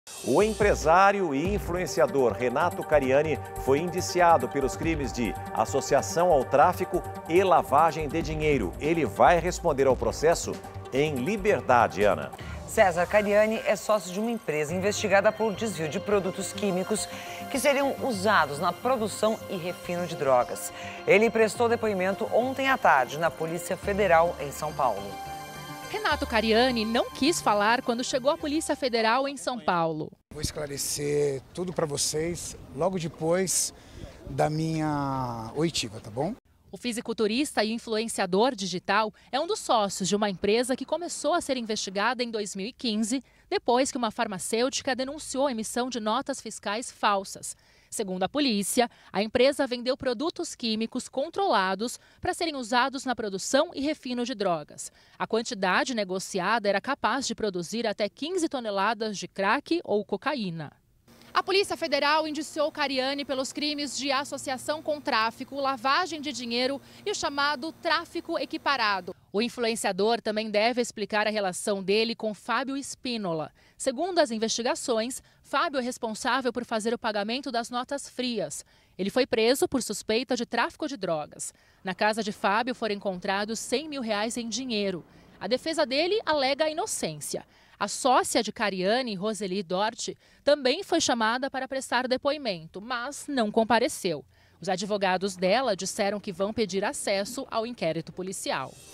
Renato Cariani é indiciado por associação ao tráfico e lavagem de dinheiro
O empresário e influenciador Renato Cariani foi indiciado pelos crimes de associação ao tráfico e lavagem de dinheiro. Ele vai responder ao processo em liberdade. Cariani é sócio de uma empresa investigada por desvio de produtos químicos que seriam usados na produção e refino de drogas. Ele prestou depoimento ontem à tarde, na Polícia Federal, em São Paulo.